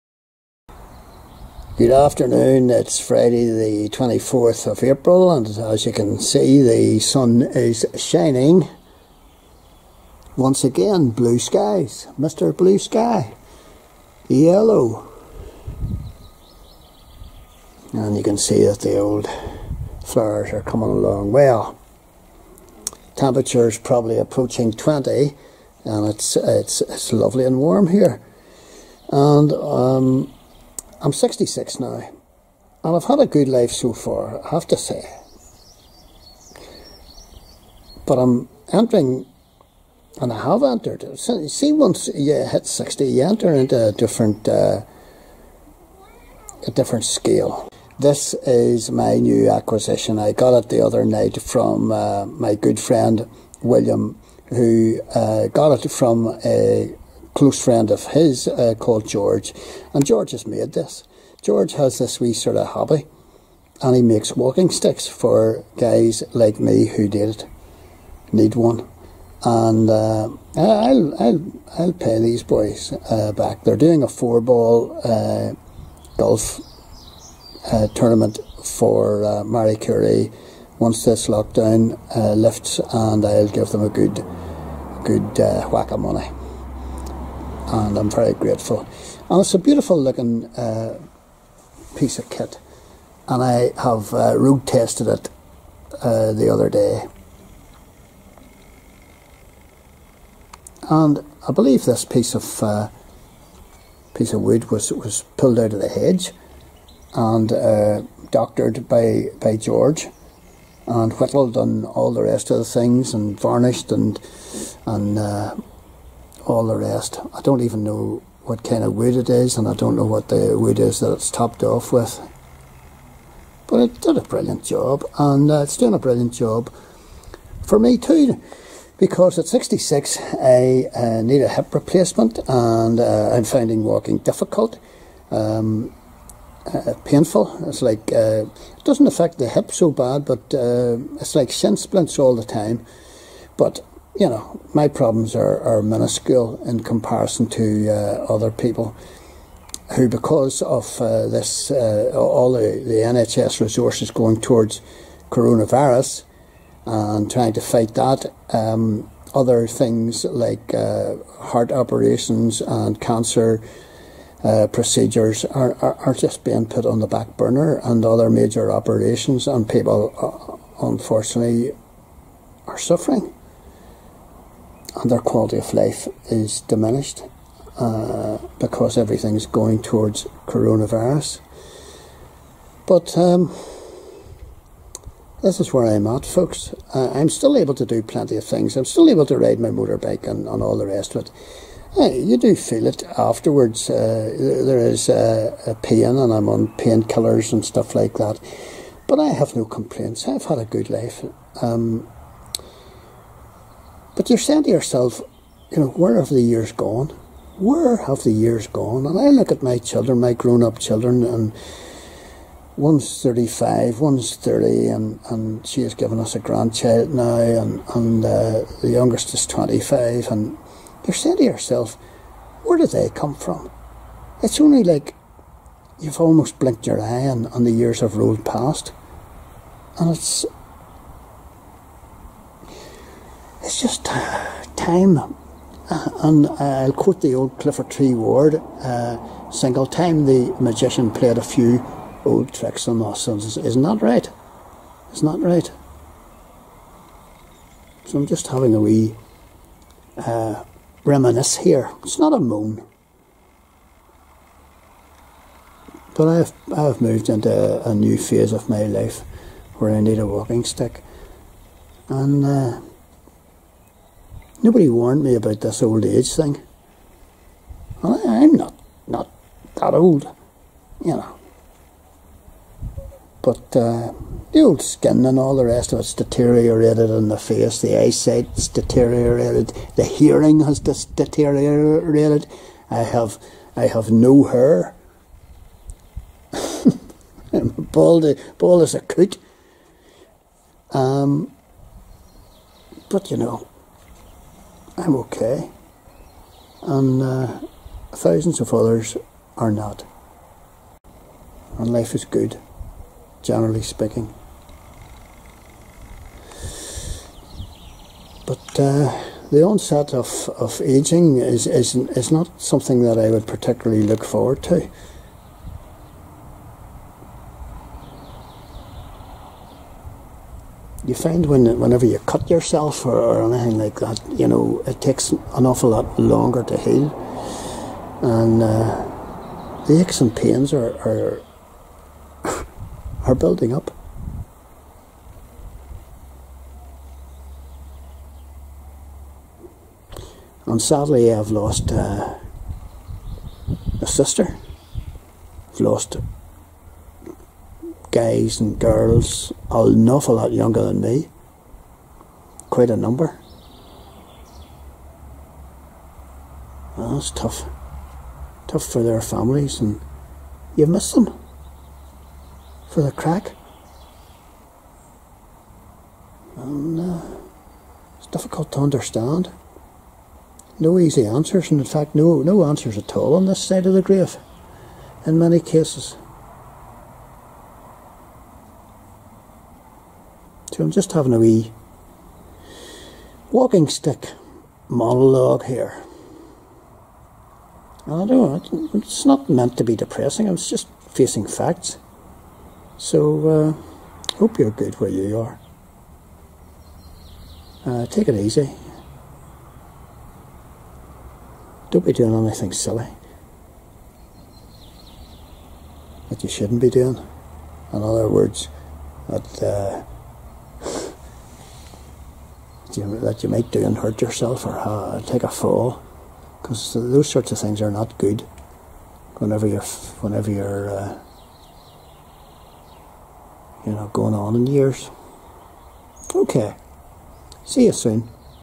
My New Walking Stick/Aging Monologue
Introducing my new ever ready, 'friend',. - my new walking stick.